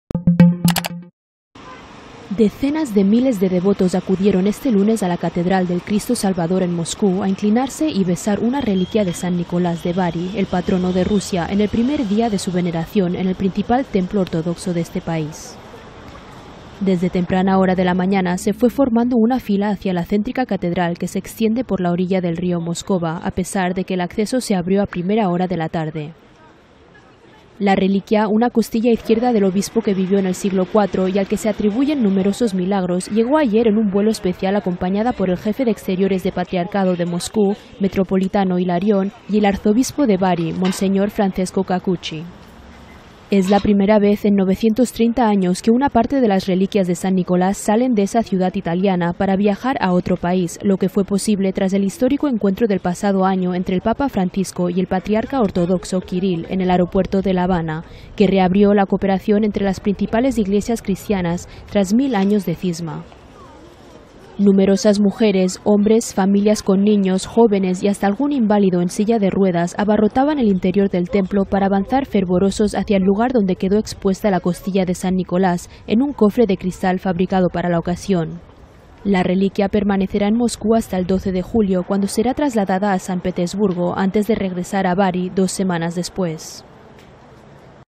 Miles de devotos ortodoxos se inclinan ante reliquia de San Nicolás en Moscú
Moscú (Rusia), 22 may (EFE),- (Imagen:Bernardo Suarez).- Decenas de miles de devotos acudieron hoy a la catedral del Cristo Salvador en Moscú a inclinarse y besar una reliquia de San Nicolás de Bari, el patrono de Rusia, en el primer día de su veneración en el principal templo ortodoxo de este país.
IMÁGENES DE LA CATEDRAL CRISTO REDENTOR DE MOSCÚ EN RUSIA.